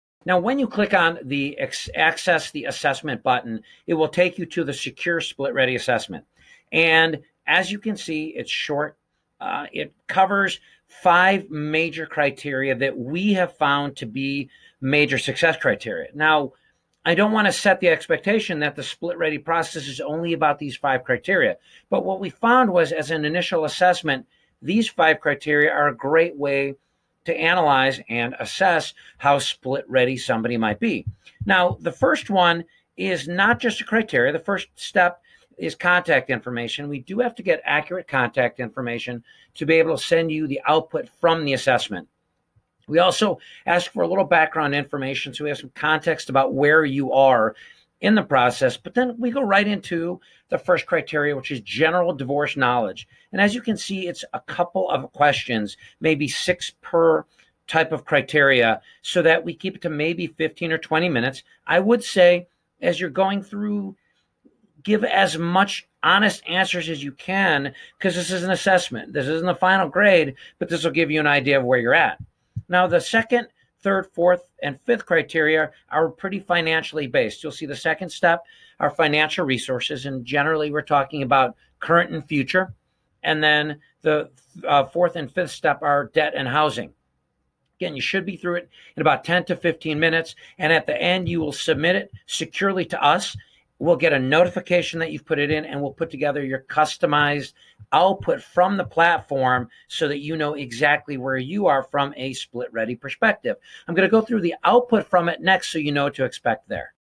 SplitReady Assessment Walk Through - The SplitReady Assessment Questionnaire
Our simple, free assessment will only take you a few minutes, but it will enable us to get you a good idea of where you currently stand on your divorce readiness.

Are you wondering if you are split ready? and take the free assessment.

Are you wondering if you are split ready, take our free, no cost assessment